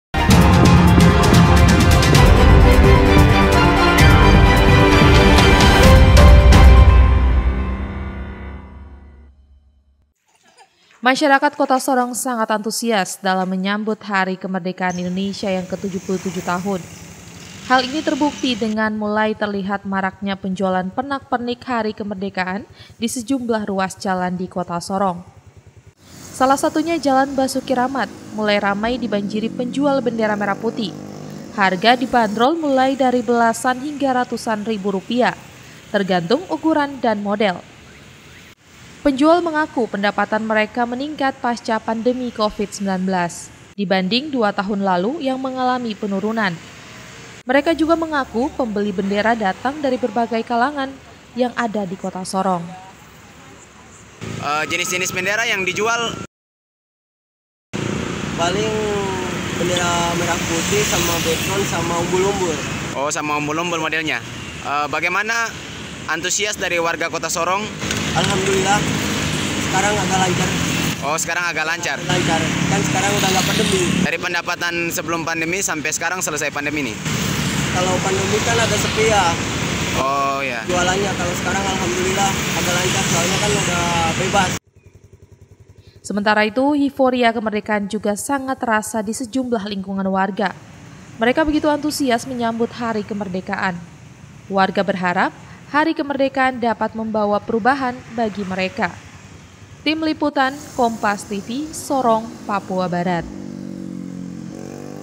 Warga Sorong Mulai Hiasai Lingkungan Dengan Pernak Pernik Kemerdekaan
SORONG, KOMPAS.TV - Masyarakat Kota Sorong sangat antusias dalam menyambut hari Kemerdekaan Indonesia yang ke 77 tahun. Hal ini terbukti dengan mulai terlihat maraknya penjualan pernak pernik hari kemerdekaan di sejumlah ruas jalan.

Salah satunya jalan Basuki Rahmat mulai ramai dibanjiri penjual bendera merah putih. Harga dibanderol mulai dari belasan hingga Ratusan Ribu Rupiah, tergantung ukuran dan model.

Penjual mengaku pendapatan mereka meningkat pasca pandemi Covid 19, dibanding 2 tahun lalu yang mengalami penurunan. Mereka juga mengaku pembeli bendara datang dari berbagai kalangan, yang di Kota Sorong.

Sementara itu hiporia kemerdekaan juga sangat terasa di sejumlah lingkungan warga, mereka begitu antusias menyambut hari kemerdekaan. Warga berharap dapat membawa perubahan bagi mereka.